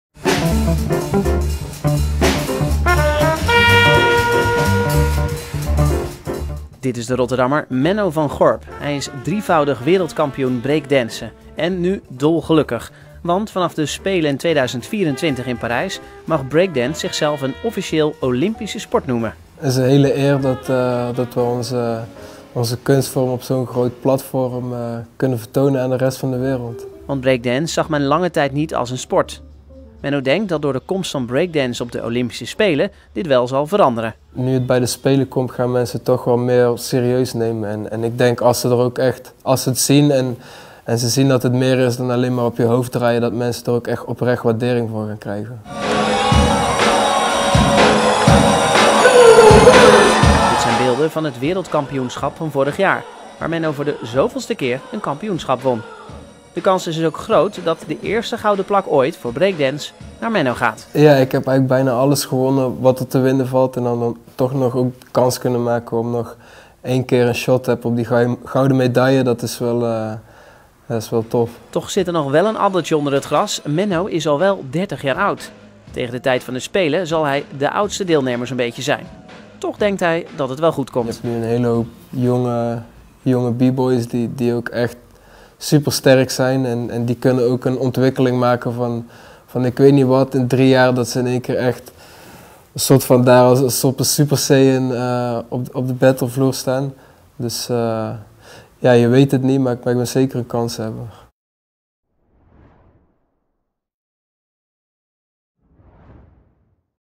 Van Gorp wil voor gouden plak gaan tijdens de Spelen van 2024 in Parijs.
Breakdancer en driemalig wereldkampioen Menno van Gorp (31) kan zijn geluk niet op, want breakdance debuteert als officiële sport bij de Olympische Spelen van 2024 in Parijs. De competitieve vorm van breakdance &apos;breaking&apos; wordt aan het programma toegevoegd, maakte het Internationaal Olympisch Comité (IOC) gisteren bekend.De Rotterdammer staat bekend als een van de beste breakdancers van het land en mag zich gaan verheugen op een nieuwe uitdaging. &quot;Het is een hele eer dat we onze kunstvorm op zo&apos;n groot platform kunnen vertonen aan de rest van de wereld&quot;, zegt hij.[article:188233:Rotterdamsebreakdancerklaarvoorderdewereldtitel]  Het IOC kwam tot deze beslissing, nadat de danssport vorig jaar al officieus werd gepresenteerd als sport. Hierdoor hopen ze een jonger publiek aan te spreken. Ook skateboarden, klimmen en surfen zijn toegevoegd aan het programma. &apos;Hele mooie motivatie&apos;&quot;Voor mij persoonlijk is het een hele mooie motivatie om veel te trainen&quot;, zegt de in Rotterdam-Zuid opgegroeide breakdancer. Met zijn 31 jaar oud ziet hij meedoen aan de Spelen als &apos;ongeveer zijn eindhalte&apos;. &quot;Het is heel mooi als je dit aan het eind van je carrière nog mag meemaken.&quot;[article:174795:WereldkampioenbreakdancengeinspireerddoorRotterdam-Zuid] Het is een nieuwe uitdaging voor Van Gorp, want verder heeft hij bijna alles gewonnen wat er te winnen valt. Zo won hij als enige persoon ooit drie keer de Red Bull BC One, de meest begeerde titel in de breakdancewereld. Ook zette hij in 2019 de derde wereldtitel op zijn naam. Hij wil de gouden plak pakken. &quot;Dat zou wel heel erg tof zijn. Ik zie mijzelf zeker als kanshebber.&quot; Breakdance als sport Voor van Gorp is het nog wel even wennen dat breakdance nu officieel als sport wordt gezien, al spreekt hij liever over &apos;een kunstvorm&apos;. Mensen zullen het nu serieuzer nemen, denkt hij. &quot;Als ze zien dat het meer is dan alleen op je hoofd ronddraaien, gaan mensen er oprecht waardering voor krijgen. Dat gaat veel deuren openen.&quot;[iframe weg naar de Olympische Spelen is voor Van Gorp al begonnen. Niet dat hij nu al een move heeft bedacht, want het gaat volgens hem meer om de hele stijl. Het nadenken daarover doet hij nu al. &quot;Hoe ik daar als persoon ga staan, als danser, om mijn ding zo compleet mogelijk neer te zetten.&quot; Geven en nemenKritiek bestaat er ook, over dat de danssport vercommercialiseert op zo&apos;n groot platform als de Spelen. Van Gorp beaamt dat. Veel grote competities worden live uitgezonden, waardoor breakdancers vastzitten aan de rechten van de muziek. &quot;Breakdancers houden ervan om bijvoorbeeld op een nummer van James Brown te &apos;battlen&apos;. Dat kan dan niet gedraaid worden, omdat ze die rechten niet hebben.&quot; De breakdancers - Van Gorp zegt liever b-boys - zullen te maken krijgen met meer regels, wat even wennen zal zijn. &quot;Maar het is geven en nemen&quot;, benadrukt hij.Want de Spelen bieden ook mogelijkheden. Mensen als Van Gorp hebben de kans om de cultuur van breakdance zo goed mogelijk te representeren. &quot;Daar voel ik ook echt een roeping in.&quot;